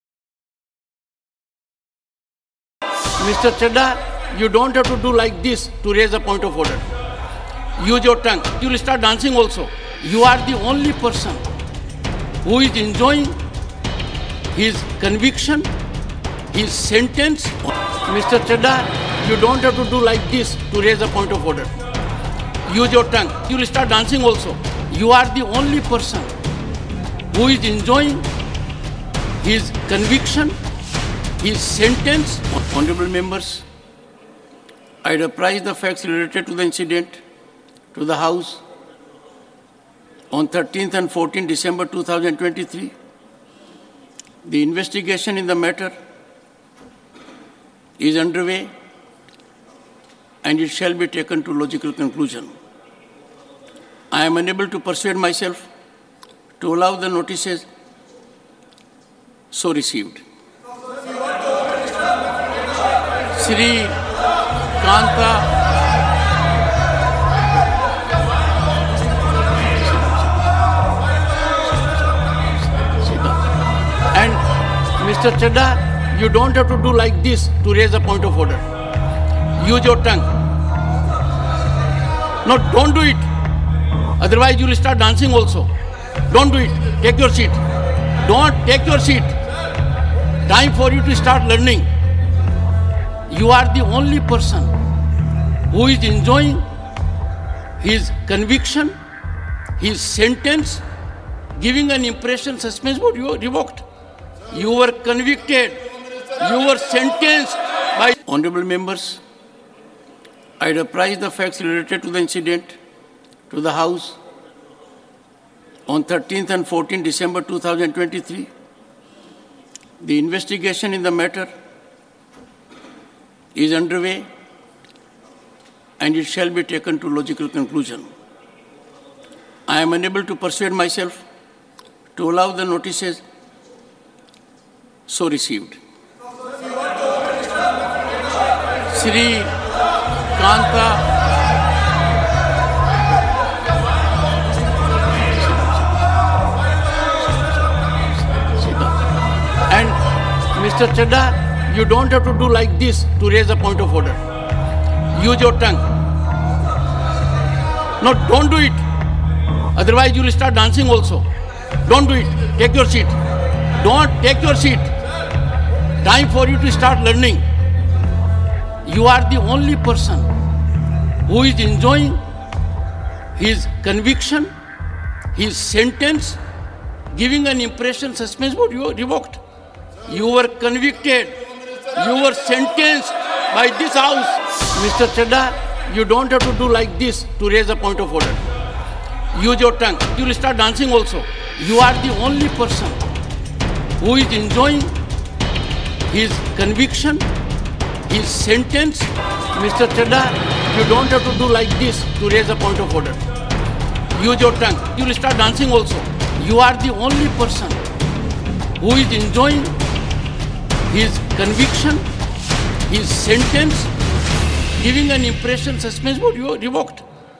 Jagdeep Dhankhar schools Raghav Chadha in Parliament | কিয় খং উঠিল ৰাঘৱ চড্ডাৰ ওপৰত অধ্যক্ষৰ? N18V
Jagdeep Dhankhar schools Raghav Chadha in Parliament | কিয় খং উঠিল ৰাঘৱ চড্ডাৰ ওপৰত অধ্যক্ষৰ?
অধিৱেশন চলি থকাৰ মাজতে জ্ৱলি পকি উঠিল জগদীপ ধনখড়
কিয় খং উঠিল ৰাঘৱ চড্ডাৰ ওপৰত অধ্যক্ষৰ?
'মুখখন ব্যৱহাৰ কৰক' খঙতে ক'লে অধ্যক্ষই
'আপুনি চোন নাচিবলৈ আৰম্ভ কৰিব অলপ পাচত'